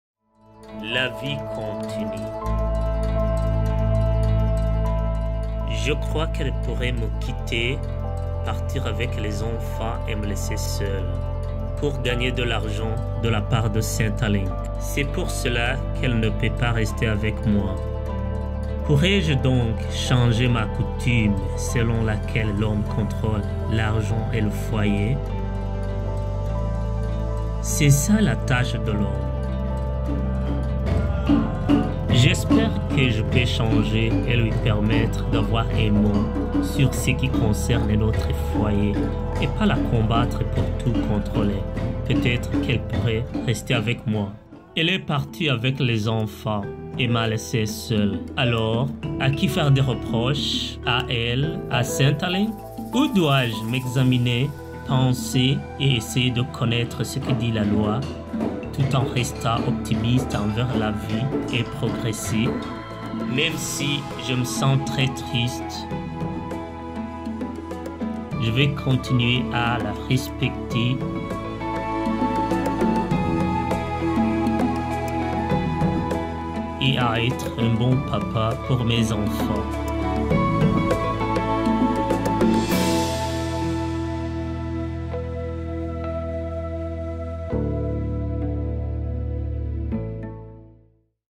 MOVE ON - French - men staying strong and respectful after separation
Why does the man think his partner will leave him? What other reasons might there be?  What is a fair way to manage money in a relationship? How can we stay respectful after family breakdown? How can people manage their deep sadness after separation?  Where can we learn more?

Versions in English, Amharic, Dinka, French, Kirundi, Somali, Sudanese Arabic, Swahili, Oromo and Tigrinya on RAILS You Tube Channel.

Men Managing Change  - family peace-building messages made in Men’s Sheds.  The videos emerged out of a two-year conversation with newly settled communities about preventing family violence and workshops exploring men's understanding of their rights, cultural attitudes and change.

The project found that many don’t know the full legal meaning of family violence, some accept family violence as normal, and some men see the system as threatening their manhood and favouring women.

Created by RAILS and South Community Hub (SCH) with support from specialist family violence agencies.  for more legal education resources.